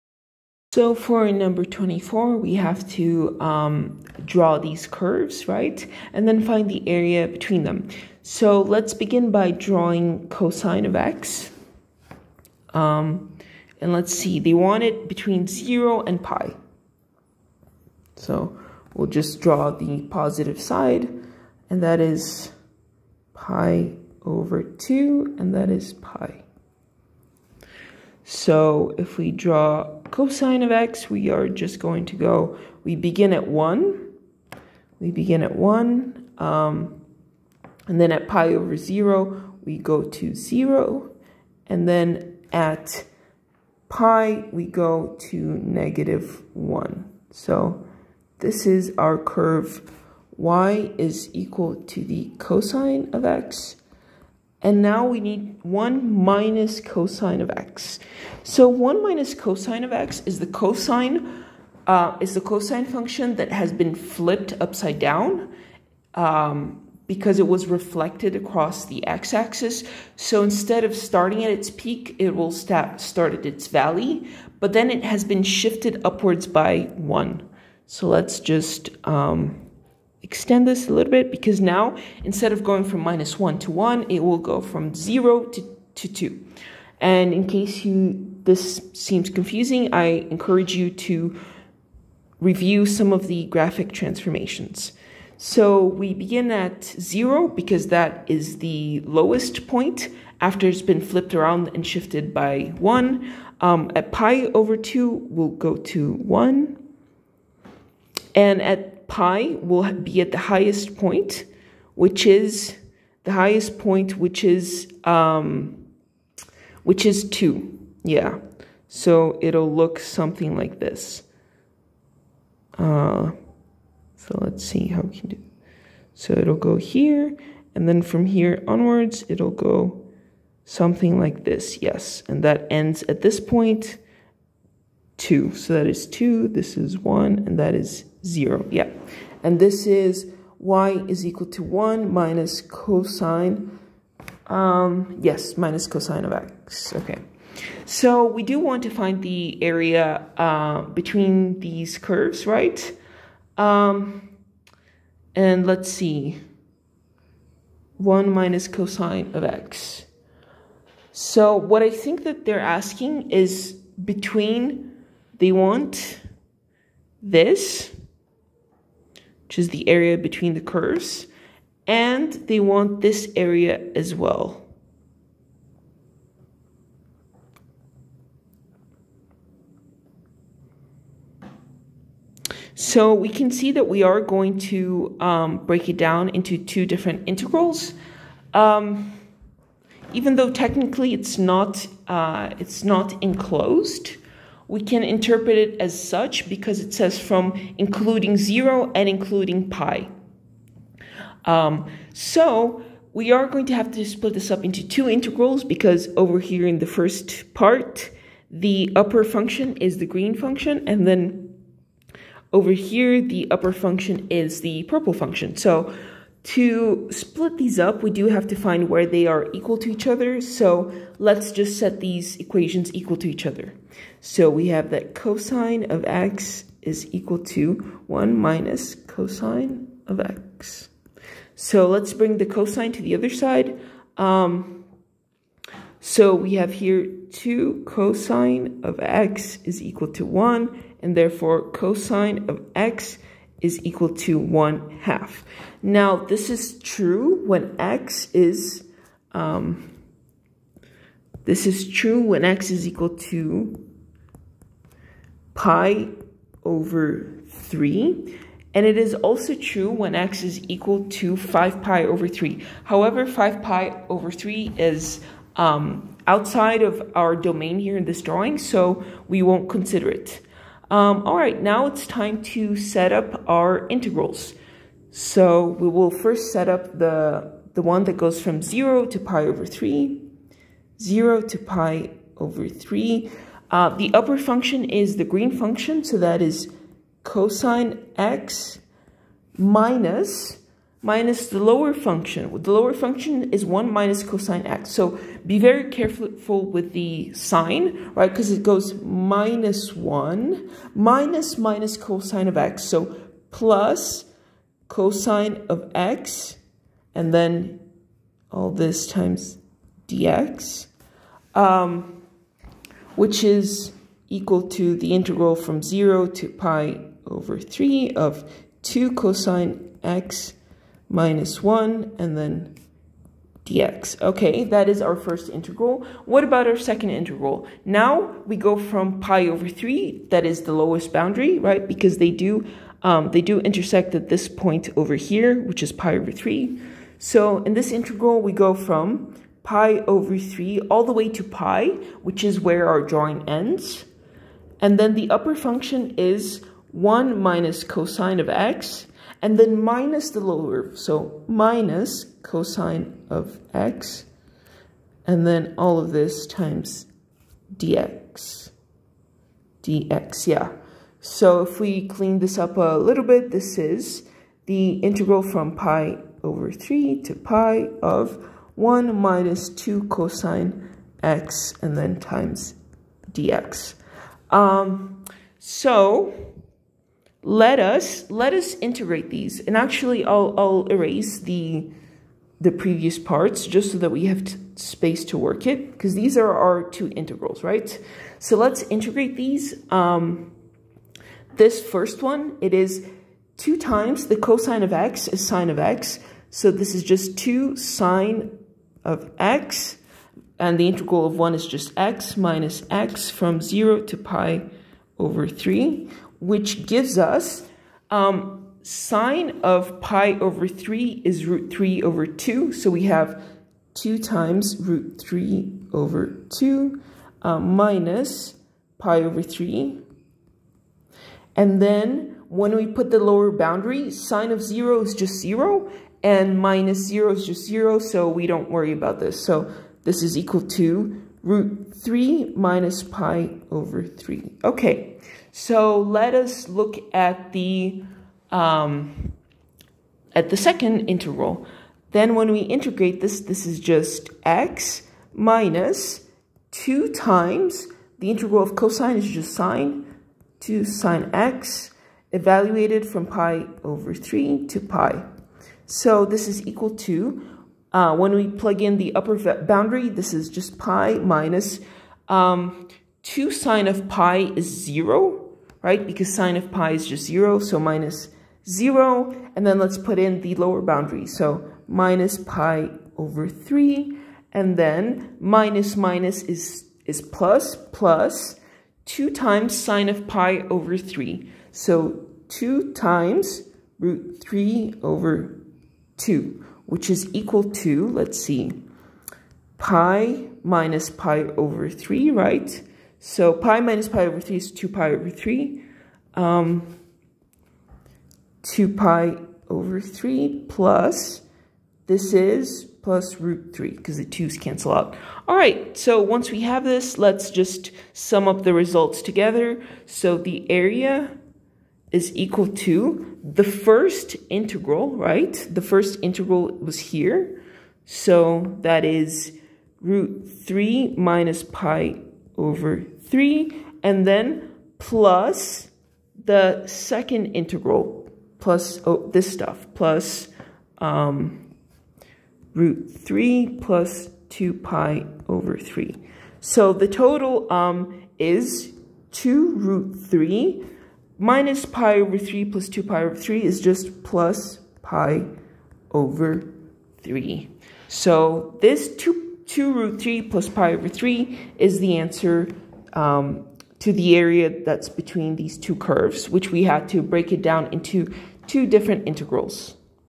6.1.24 Sketch the region enclosed by the given curves and find its area. y = cos(x), y = 1 - cos(x)
Problem 6.1.24 From James Stewart's Single Variable Calculus - Early Transcendentals 7th edition from chapter 6, applications of integration - areas between curves.

Sketch the region enclosed by the given curves and find its area. y = cos(x), y = 1 - cos(x)